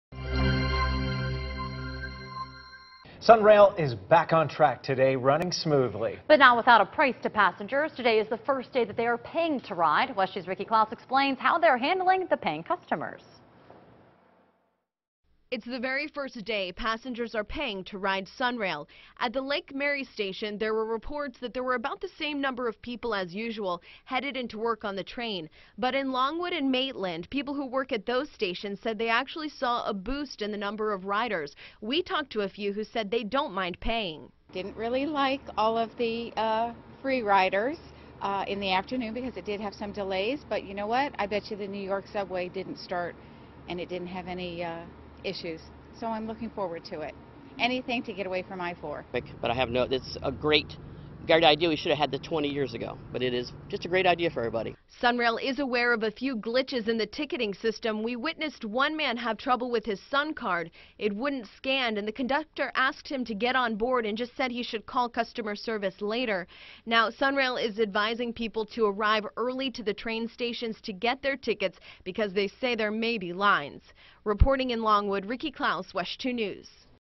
SunRail train begins charging patrons for rides
Riders will now need a SunCard or paper ticket to board SunRail trains.